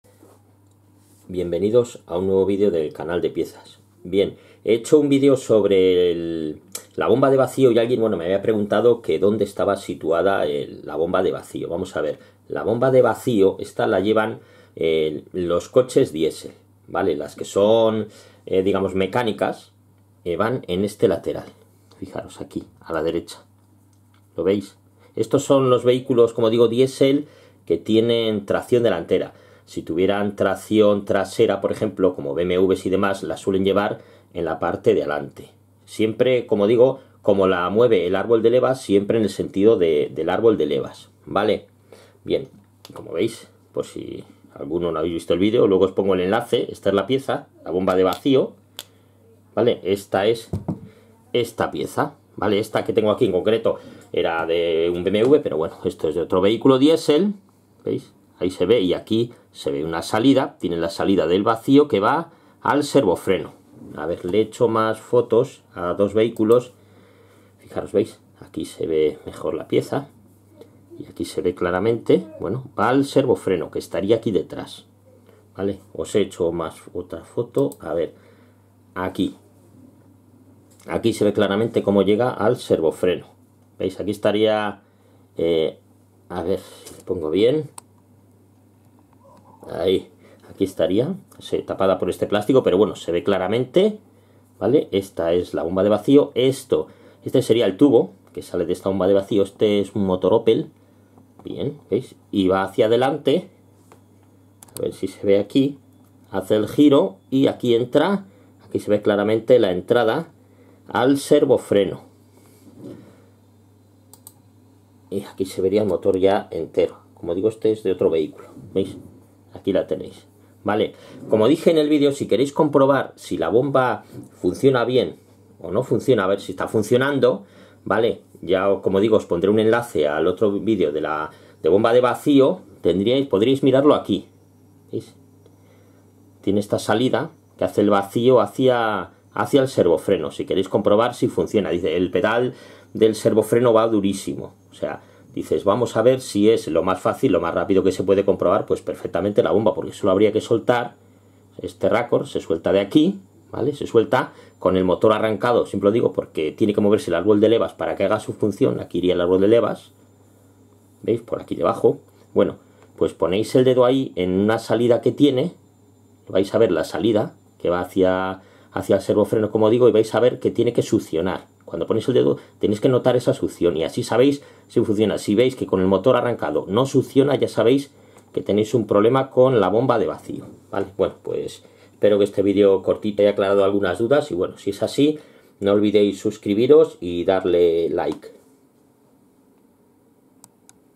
Bomba de VACIO o depresor freno, situación, comprobación y como transmite el vacio al SERVOFRENO
Bomba de vacio, depresora, como se comprueba y transmite el vacio al servofreno o booster de freno, donde está situada, depresor de freno